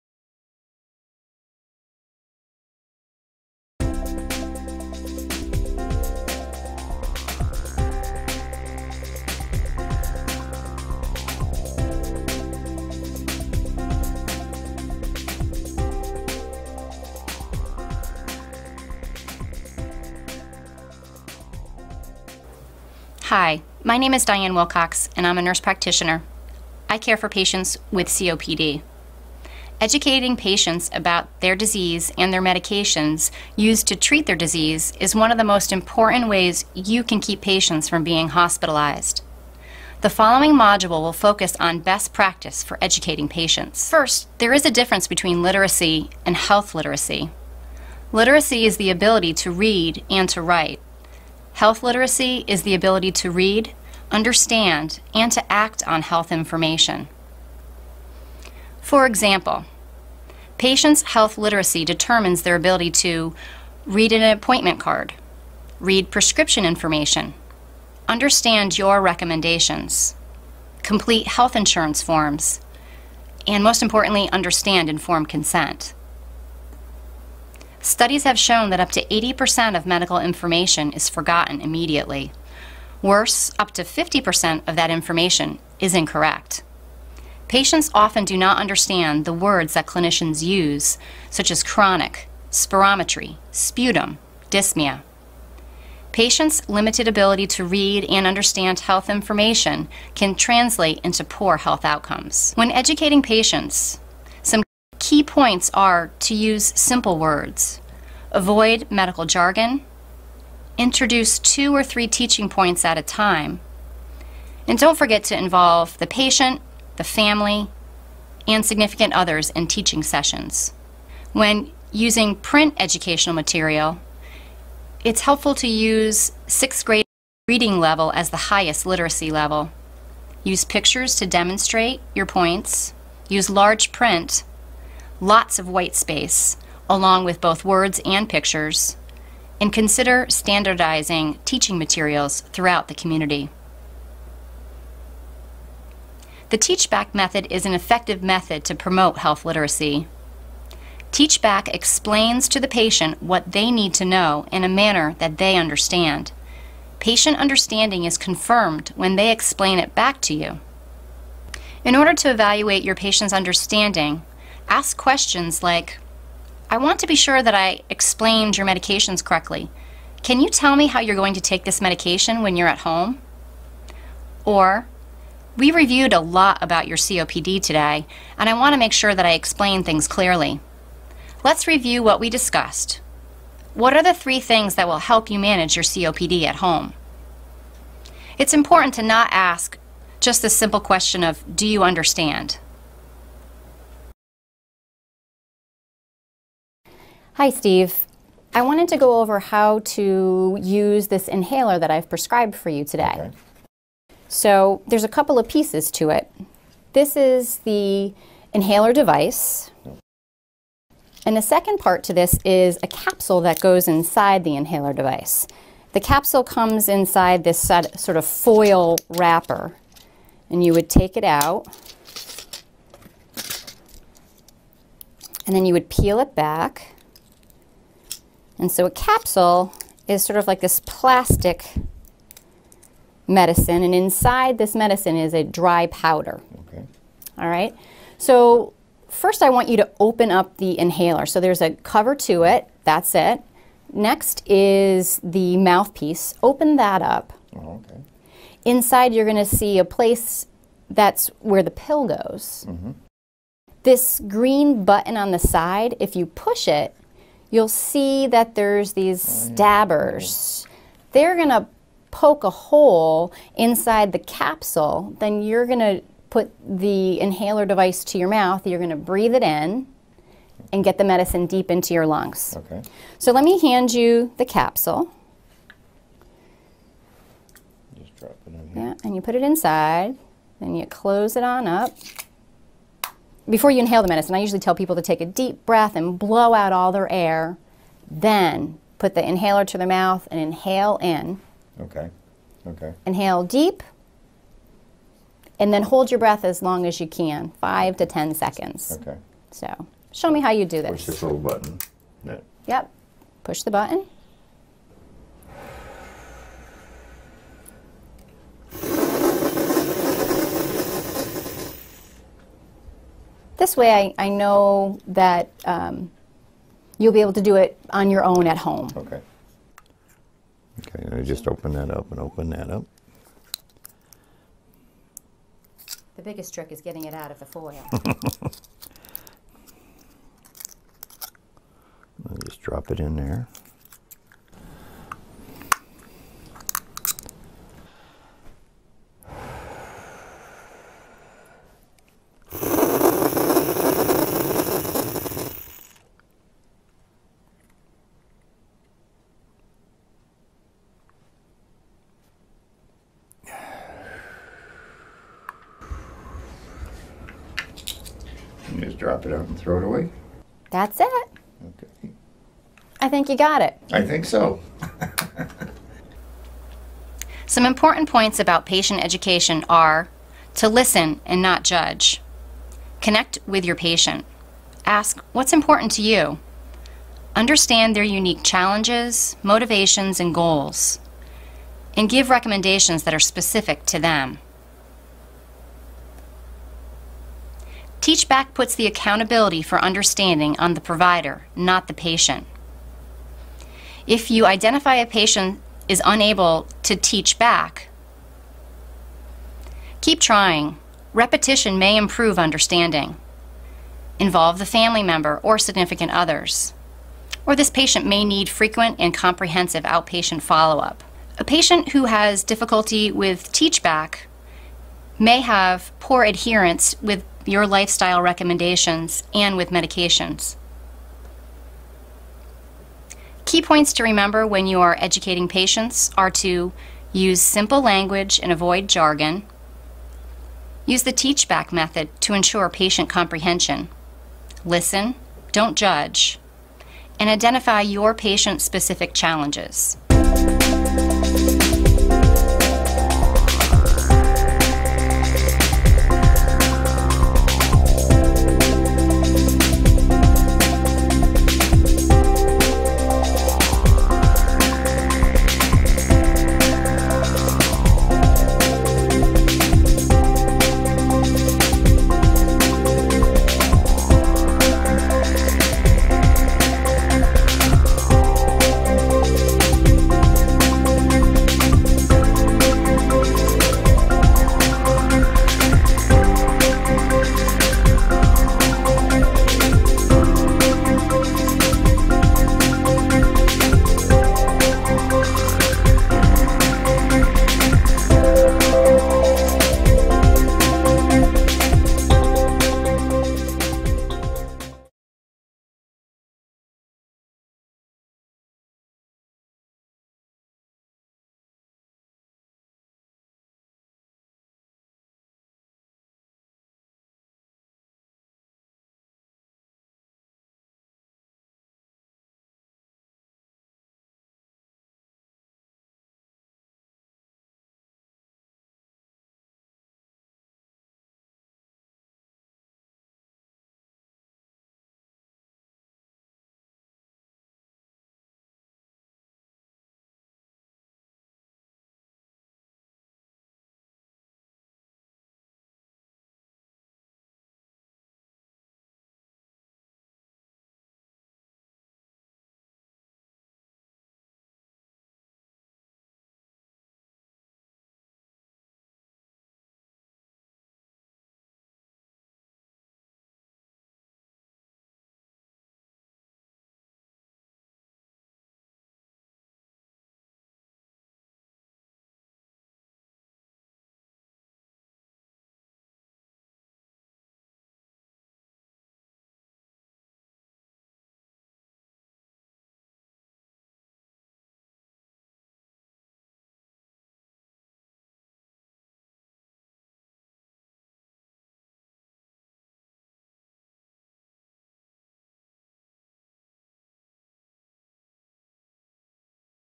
Educating Patients: LungTalk for Licensed Healthcare Professionals
Lung Talk: Teaching your patients to live with COPD is an educational video series designed for staff to review the science of COPD, provide six key recommendations, learn to educate and motivate patients, and discuss palliative and end of life care. for more information and resources.